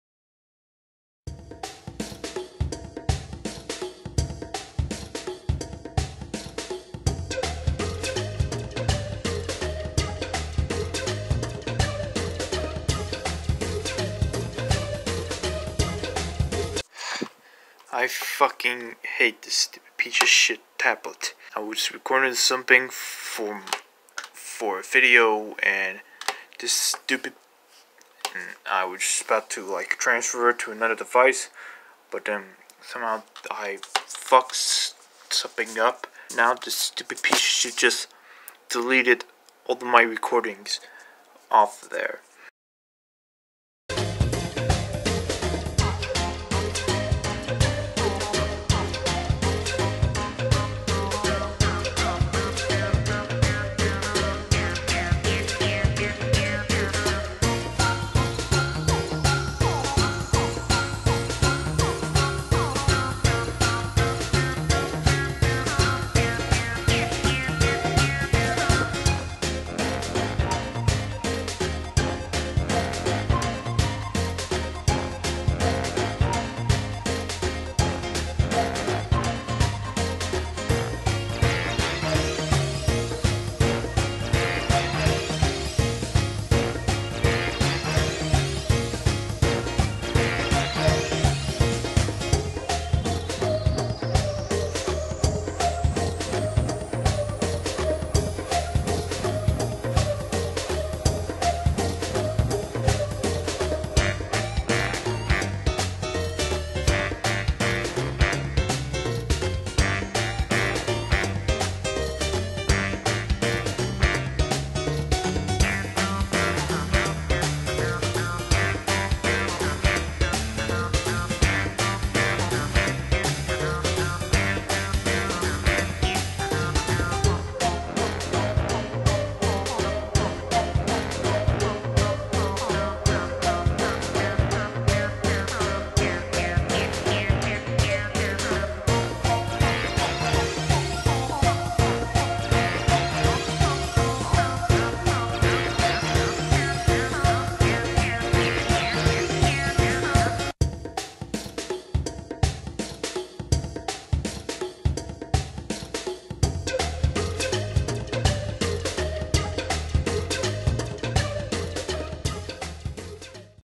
Lol this collab is canceled
This was a big mistake and I should of thought this through. But I have a different plan on November 6th

I'll be posting download links to all the sources later on.

-Frosty returns-
Minimum 0:20 Seconds long
Maximum 6:00 minuets long

-Commercials-
Minimum 10 seconds long
maximum 1:00 Minuet long